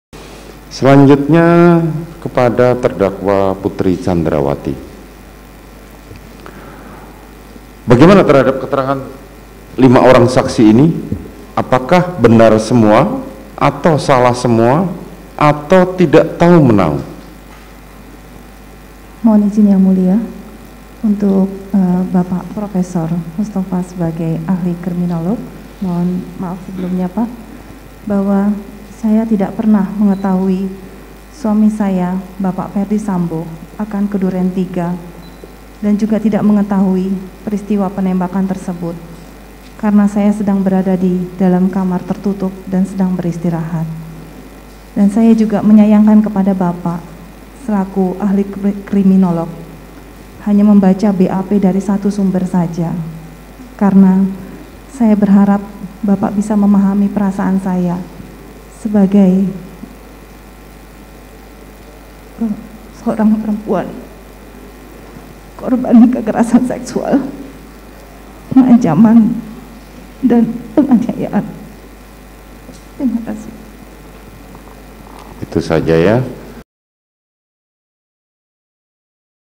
Putri Candrawathi Menangis Bantah Keterangan Ahli Kriminolog di Persidangan
JAKARTA, KOMPAS.TV - Istri Ferdy Sambo, Putri Candrawathi menangis membantah keterangan Ahli Kriminolog Muhammad Mustofa di persidangan pembunuhan Brigadir J di PN Jakarta Selatan pada 19 Desember 2022.

"Untuk Pak Mustafa Ahli Krimonolog maaf sebelumnya Pak saya tak pernah mengetahui soal kejadian di Duren Tiga dan saya tak mengetahui penembakan tersebut karena berada di kamar," ujar Putri Candrawathi.

Putri juga menyanyangkan Ahli Kriminolog berpendapat hanya dari satu sumber BAP saja.

"Saya berhadap bapak memahami, saya seorang perempuan korban kekerasan seksual dan penganiayaan," ujarnya sambil menangis.

Ada 5 ahli yang dihadirkan dalam persidangan untuk 5 terdakwa kasus pembunuhan brigadir J ini.